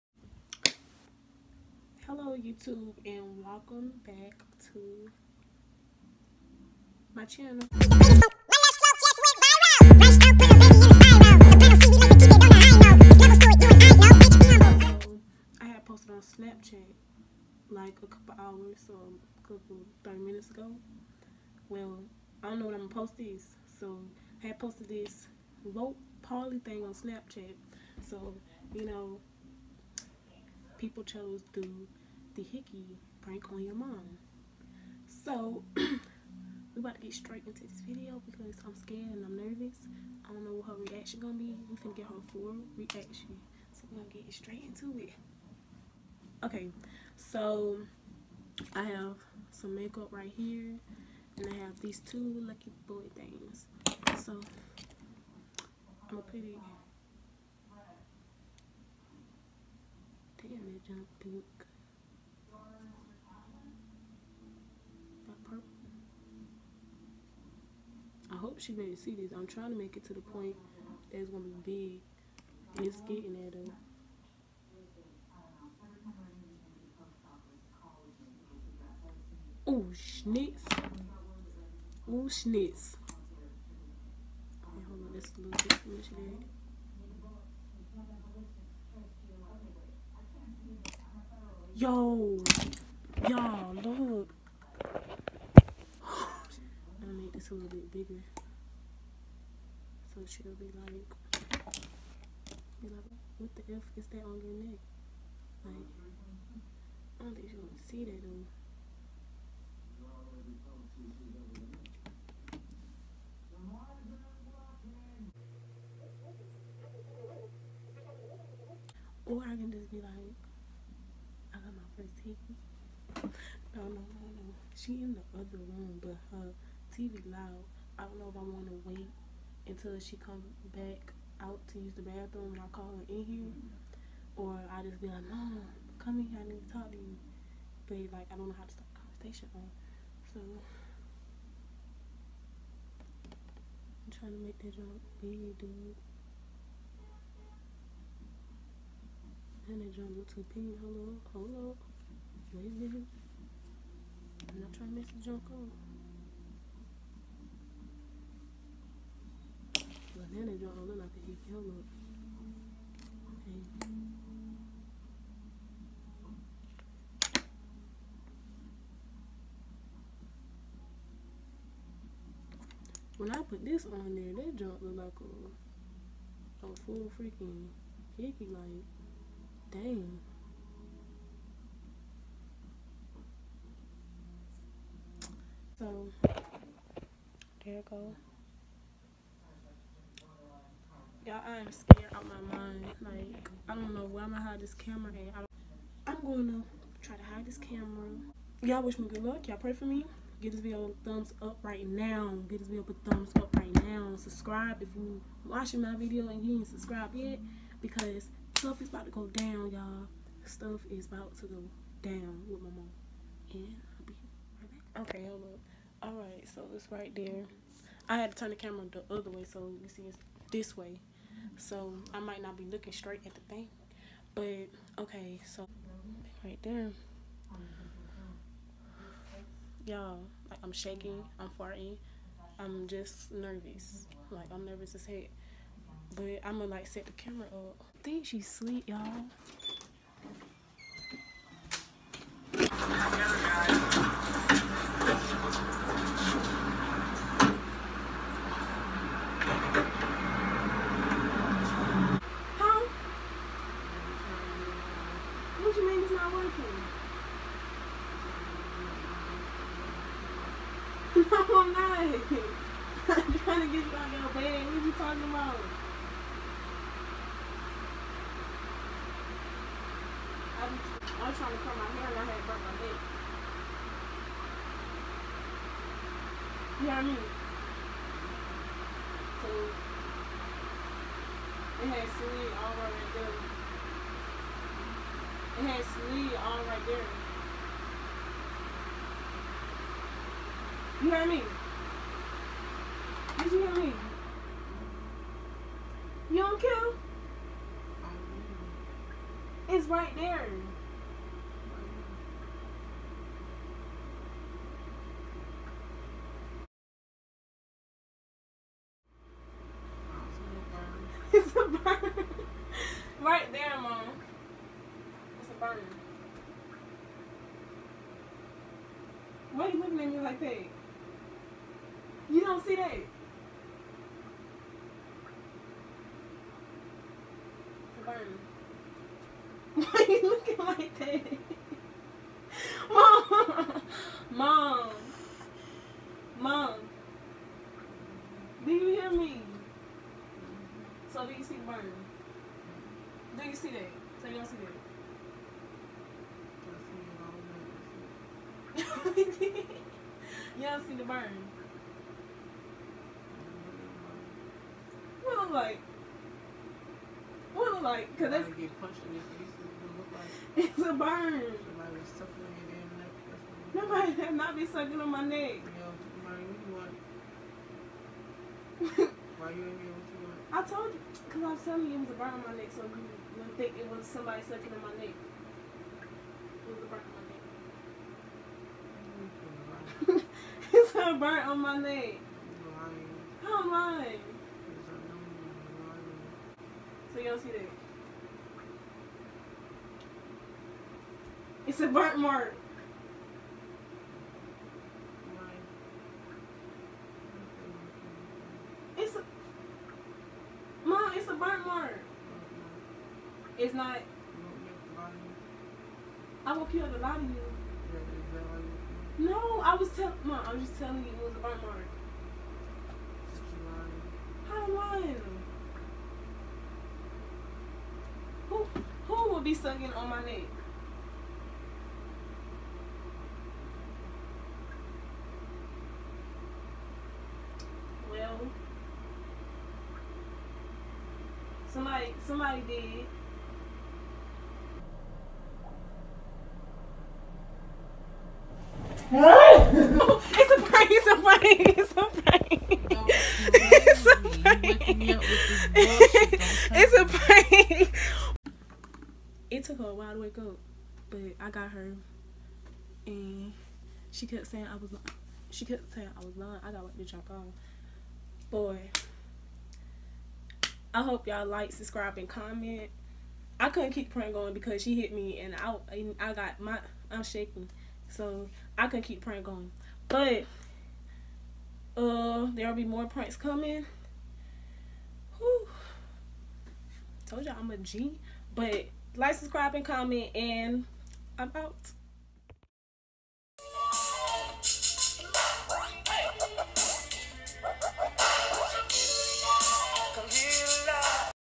Hickey prank on MOM 😱got a (REACTION) ! 🤣😂
social media ↪️ instagram @admireimani
HOPEFULLY yall can hear her I was trying to repeat everything she was saying because she was still kinda sleep 🤣 MY FIRST PRANK so go easy on me I was scared & she was sleep so I ended up going in her room PLAN had CHANGED!